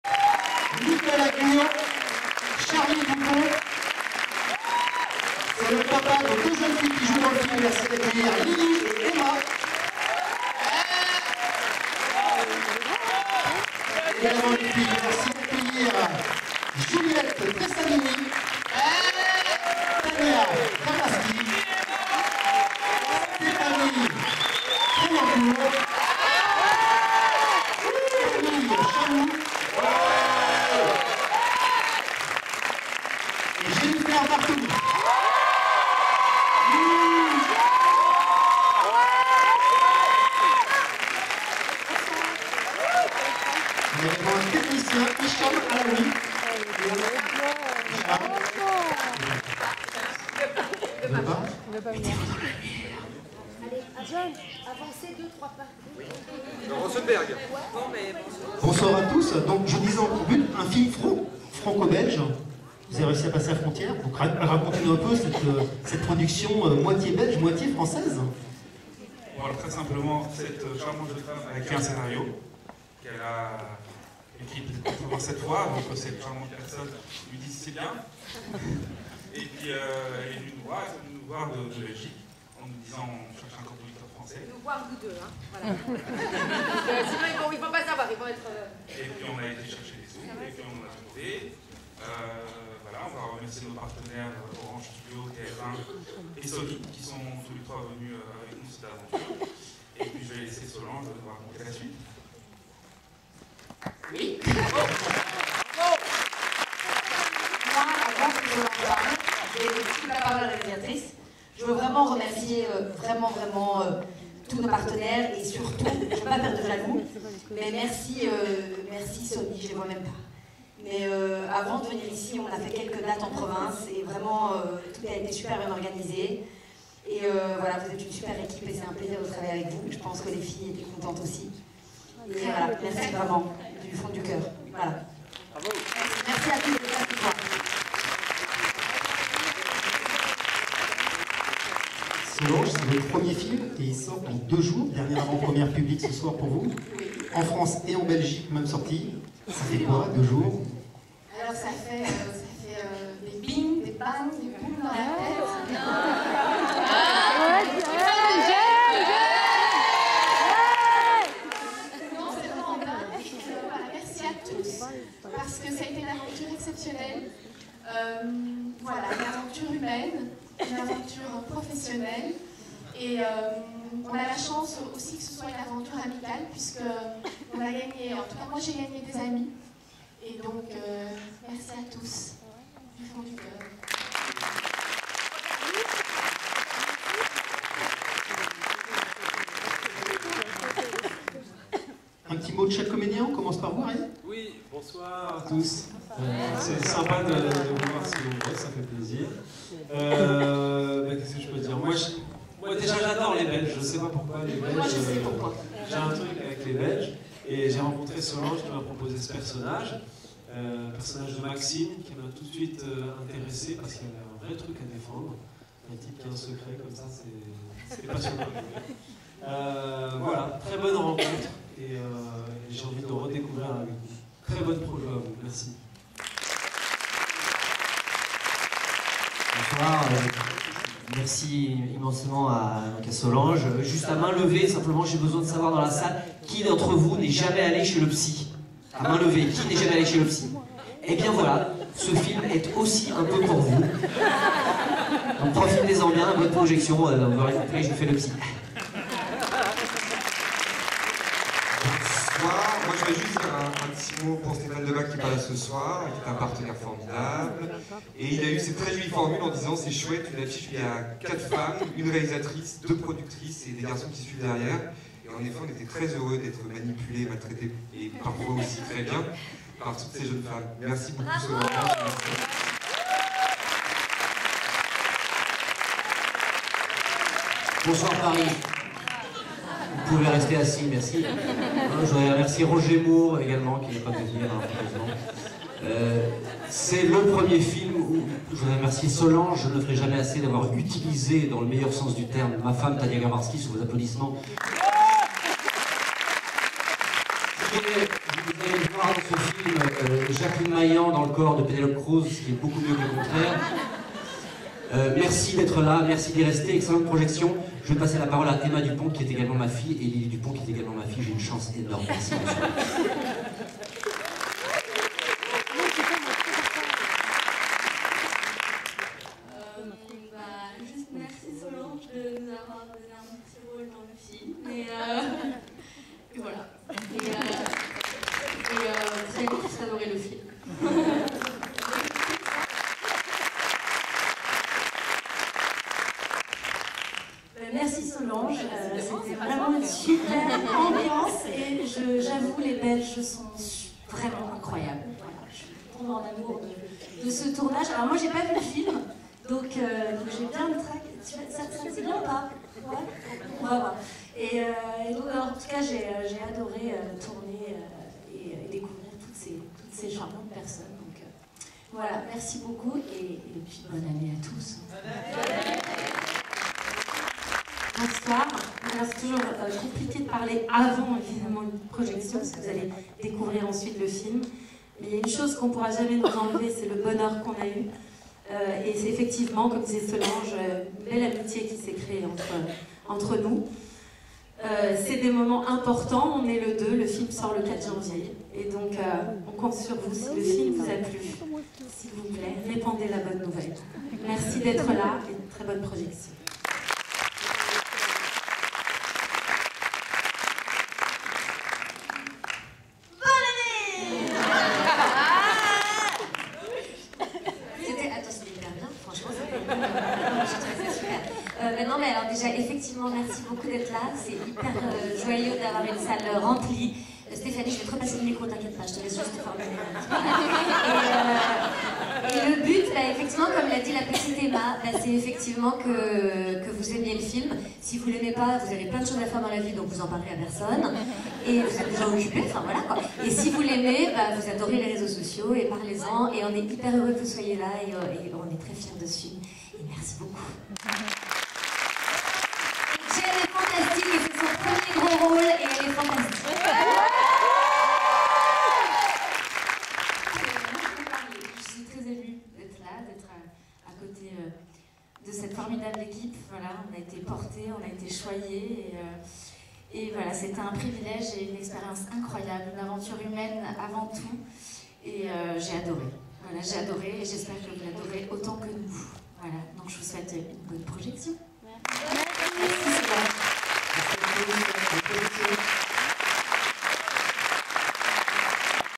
Présentation de Faut pas lui dire par l'équipe du film
Paris, UGC Ciné Cité Bercy, salle 33
Notre site était présent ce soir à l'avant-première du film Faut pas lui dire de Solange Cicurel. Les producteurs, la réalisateurs Solange Cicurel et les comédiens notamment Jenifer Bartoli, Camille Chamoux, Stéphanie Crayencour, Tania Garbarski, Arié Elmaleh sont venus à cette occasion présenter le film

Synopsis:
Laura, Eve, Anouch et Yaël sont quatre cousines, très différentes et très attachantes, qui ont un point commun : elles mentent, mais toujours par amour ! Quand les trois premières découvrent quelques semaines avant le mariage de leur petite cousine que son fiancé parfait la trompe, elles votent à l’unisson « Faut pas lui dire » !

Faut pas lui dire
Un film co-écrit et réalisé par Solange Cicurel
Avec Jenifer Bartoli (Laura Brunel),  Camille Chamoux (Eve Brunel), Stéphanie Crayencour (Yaël),  Tania Garbarski (Anouch),  Brigitte Fossey, Laurent Capelluto (Daniel Kantarian),  Stéphane Debac (David), Arié Elmaleh (Maxime Leclercq).
Distributeur : Sony Pictures Releasing France

Date de sortie; le 02/01/2017